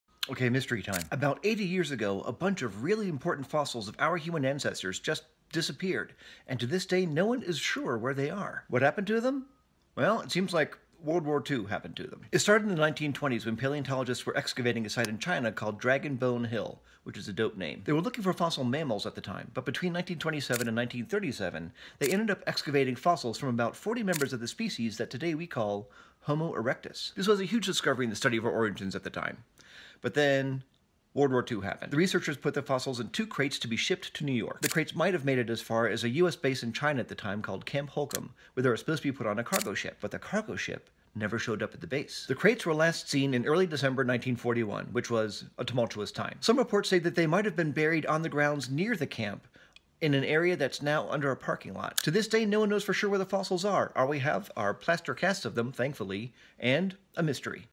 A bunch of very important fossils disappeared during WWII
80 years ago, a bunch of fossils of ancient humans disappeared.